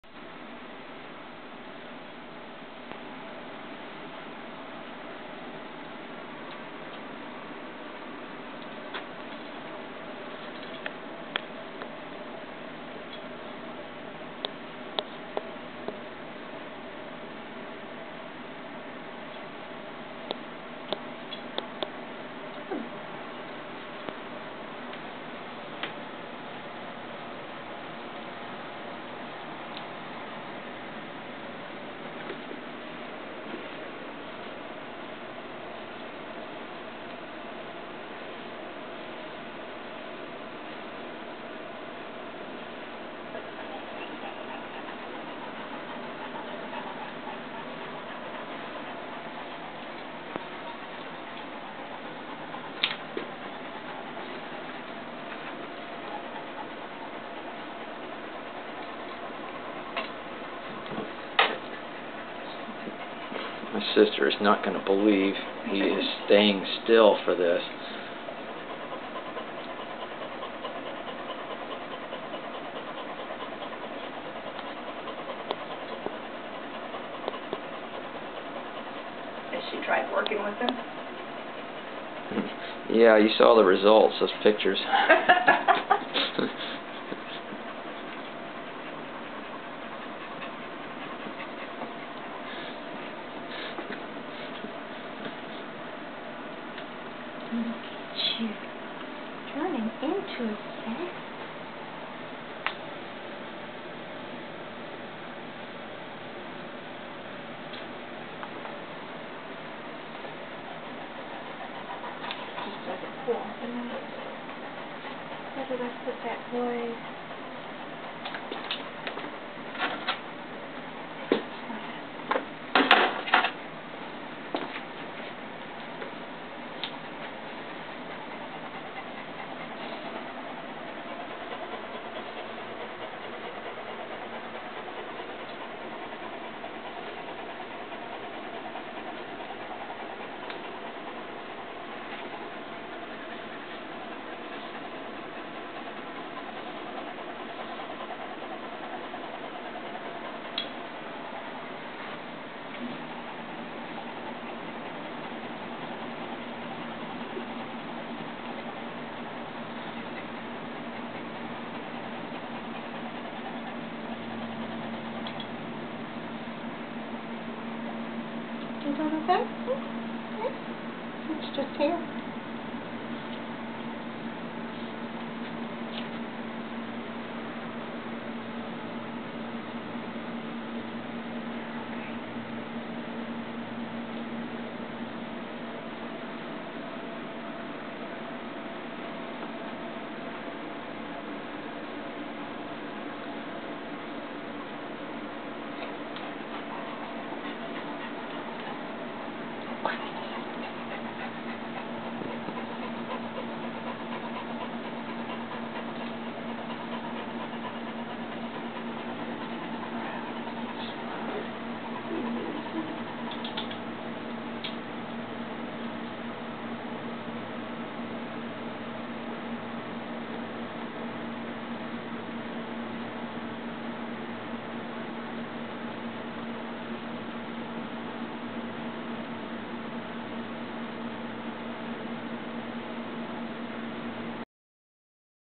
Ozzy the English Setter puppy's first grooming
Ozzy the infamous spot-monster finally gets groomed for the first time at 10 months old. It's unbeliveable how Kathy Parrott charms this little dot-headed hooligan into co-operating.